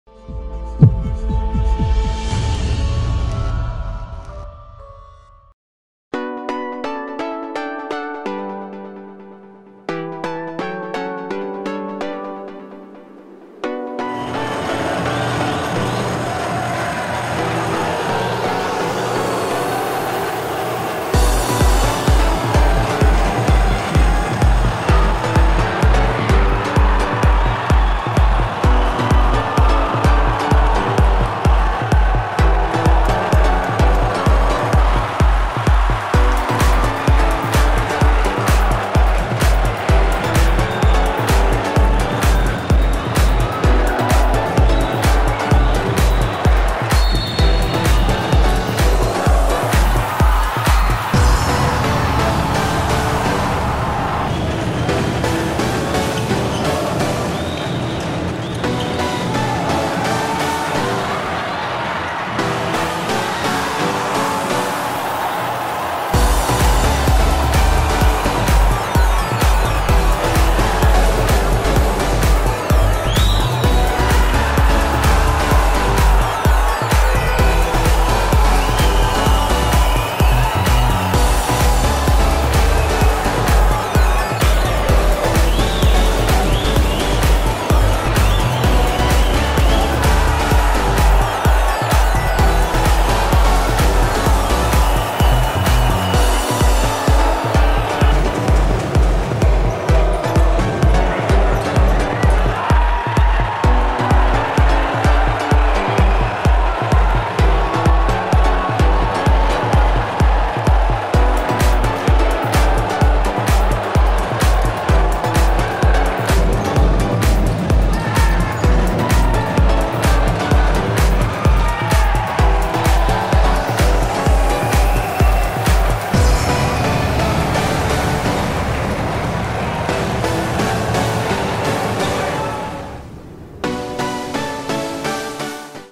LNF2019 - Gols - Semifinais Ida - Joinville 0 x 7 Magnus
Gols:
Magnus - Leozinho, Charuto (2), Danilo Baron, L. Lino, Pett e Kevin